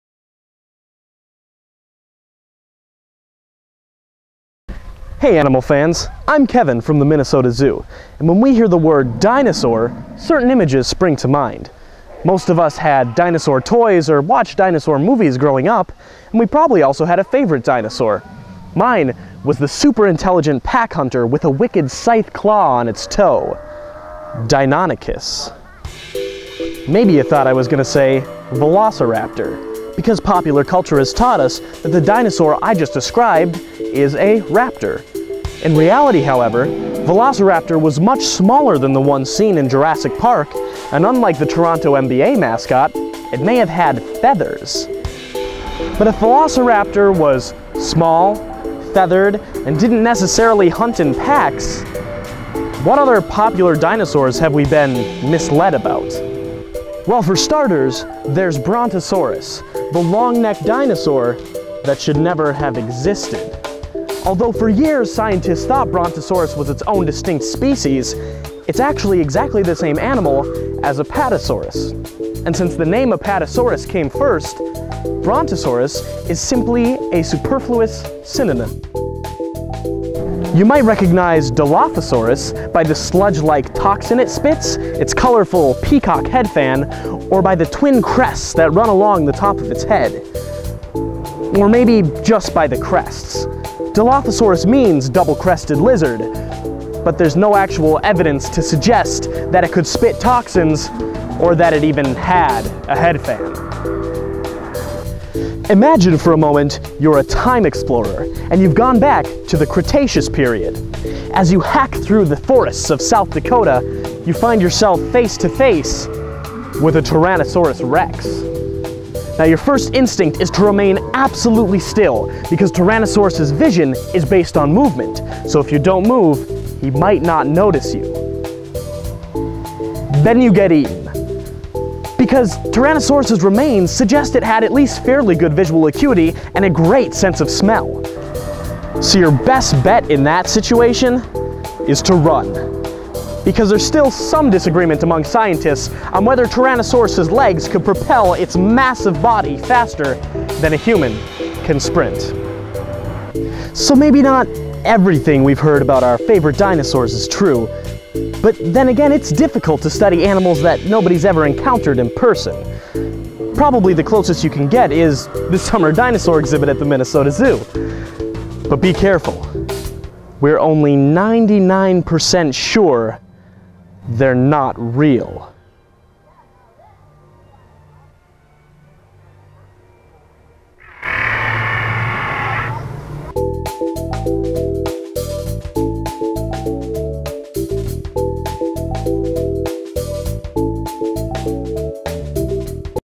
The Lowdown on Dinos
In this Minnesota Zoo video blog, we debunk some common myths about popular dinosaurs.

Sources:
Long, J.A.; Schouten, P. (2008). "Deinonychus". Feathered Dinosaurs: The Origin of Birds. Oxford University Press. pp. 142--143. ISBN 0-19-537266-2.
Barsbold, Rinchen (1983). "Carnivorous dinosaurs from the Cretaceous of Mongolia". Transactions of the Joint Soviet-Mongolian Paleontological Expedition 19: 5--119.
Minnesota Zoo Dinosaur Exhibit

Velociraptor image used under the GNU Free Documentation License.